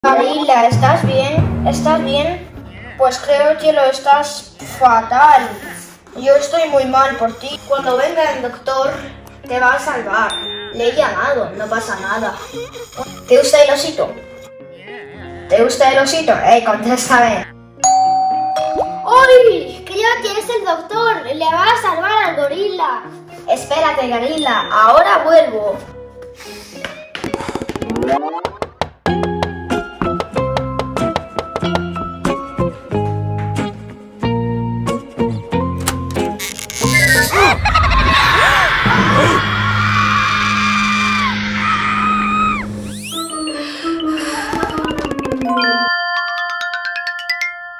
Stefan y su extraño Monstruo Doctor. Doctor Monster and gorilla funny video superhero
Stefan y su extraño Monstruo Doctor. Doctor Monster and gorilla.
Gavrila se enfermó y Stefan llamó al médico, el médico resultó ser un monstruo. Mira un video muy interesante para Halloween. cuento de hadas para niños superhero